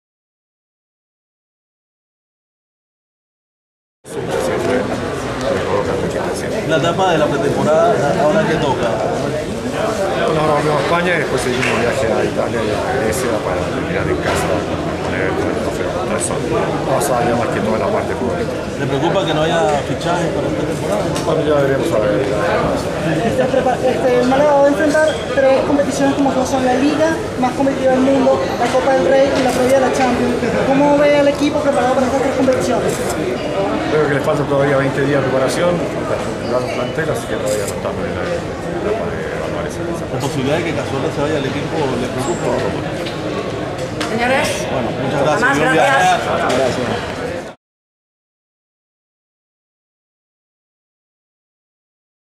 Manuel Pellegrini [DT Málaga CF] | GradaDigital.com
El estratega, Manuel Pellegrini (DT Málaga CF), conversa ante medios de prensa, una vez finalizado el encuentro ante el Caracas FC de Venezuela, con empate (2-2) - GradaDigital.com